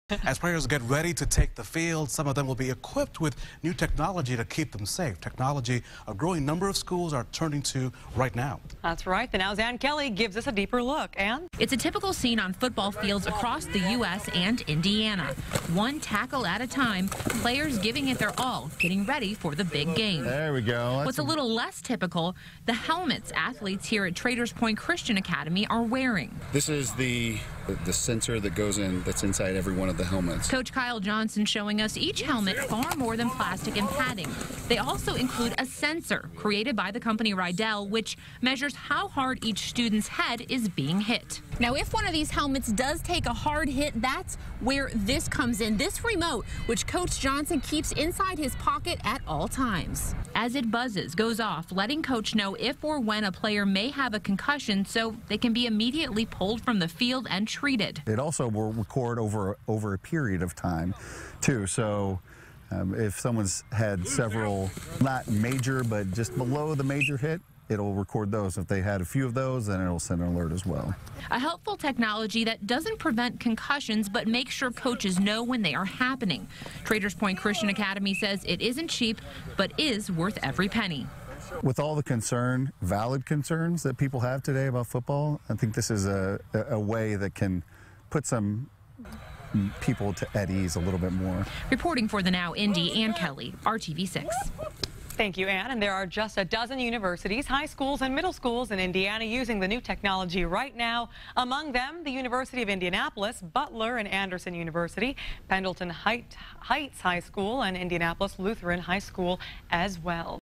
Traders Point Christian Academy finds safety success in football team helmet sensors
It's a typical scene on football fields across Indiana right now: One tackle at a time, players are giving it their all to get ready for the big game. But what's a little less typical is the helmets athletes are wearing at one local high school.